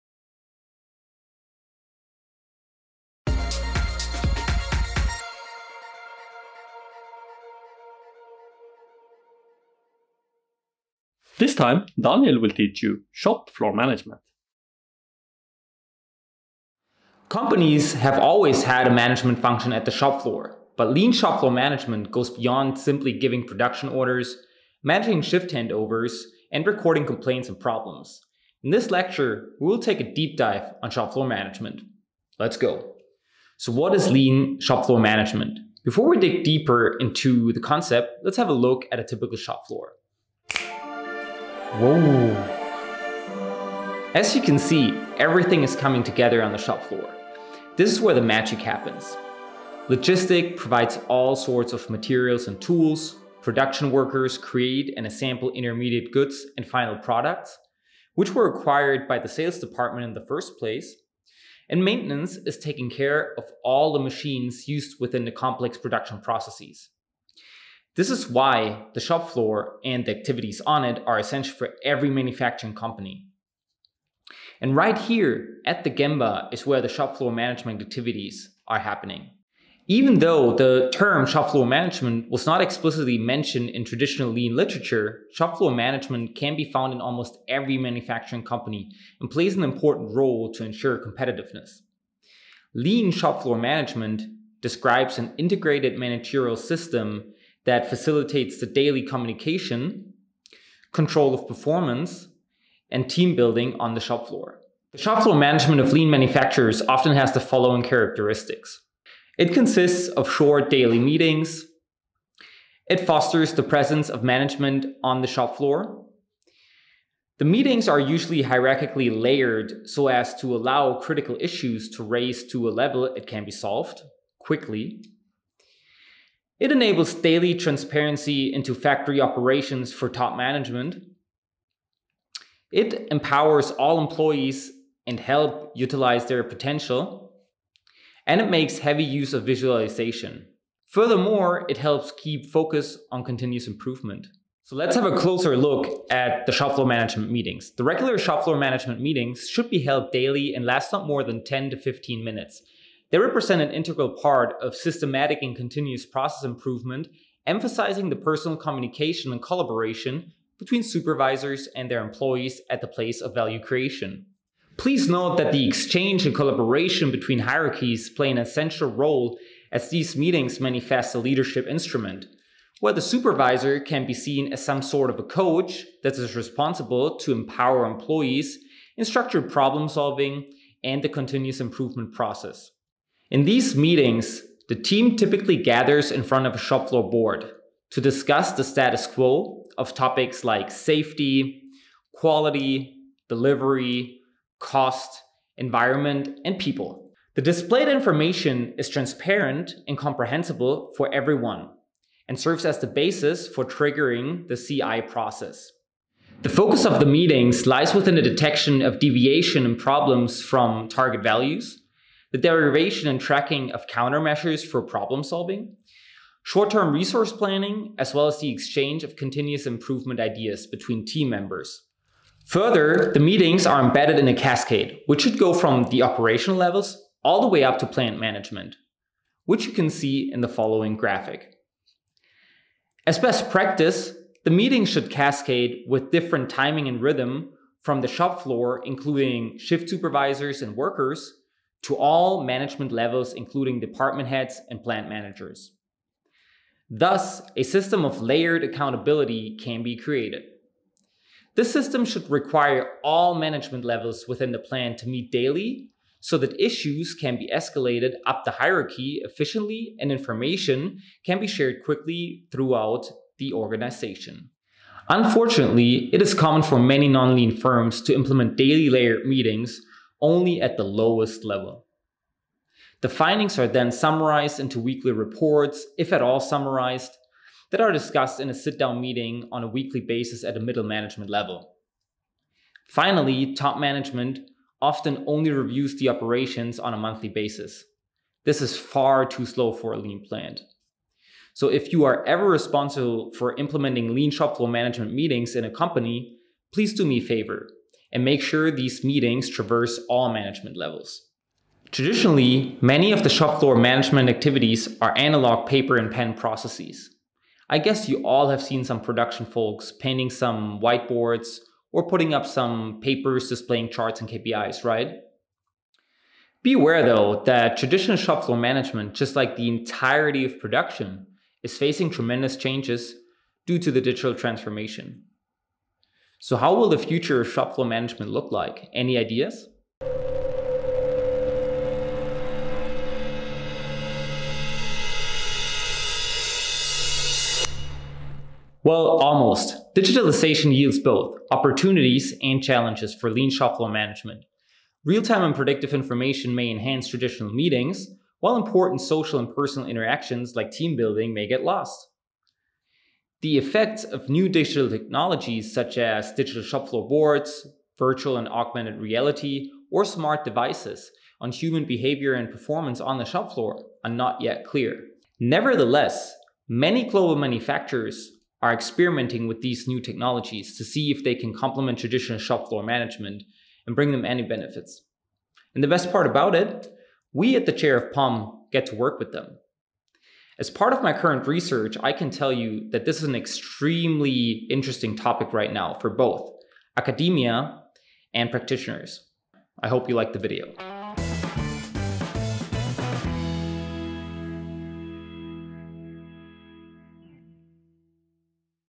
Shop Floor Management
What is Shop Floor Management? And what does it have to do with lean and digitalization?
In this lecture video, Daniel Kwasnitschka explains Shop Floor Management. The video is part of the POM4all initiative by Prof. T. Netland at the ETH Zurich. Feel free to use all POM4all videos as you please in your studies and lectures:)